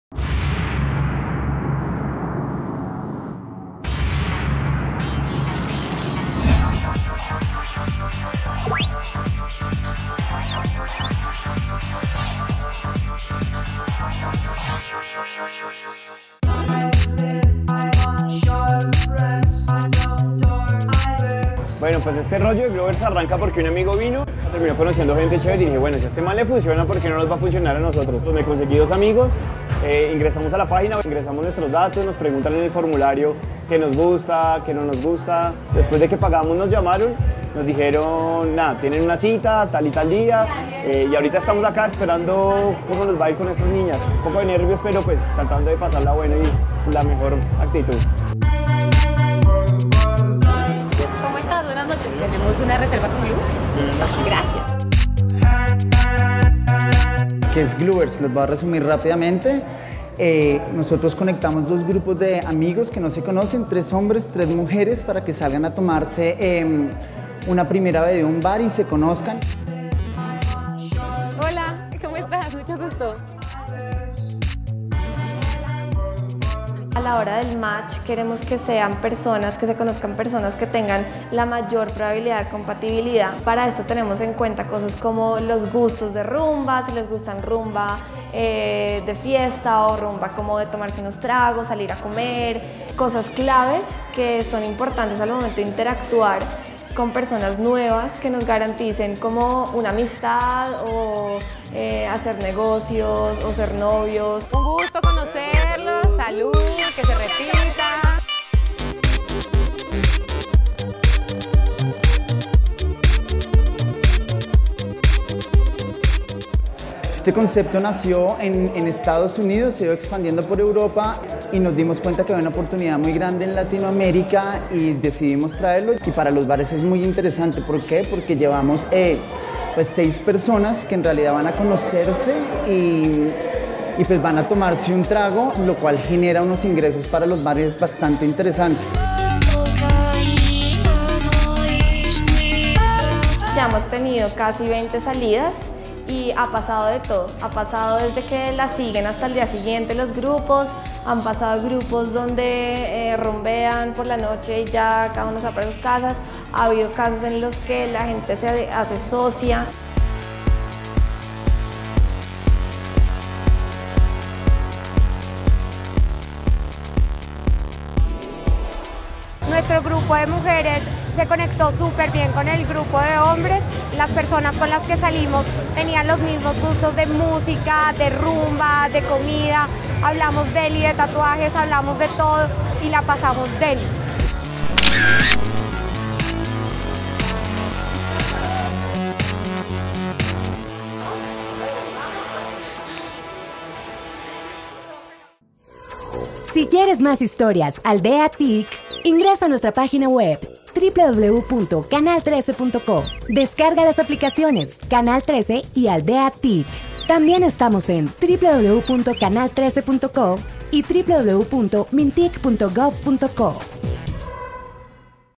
AldeaTIC Glubers
Glubers, el Tinder colombiano que reúne amigos para conocer gente nueva
Glubers es salir de la rutina, es abrir tu mundo a nuevas personas y experiencias de una forma realmente divertida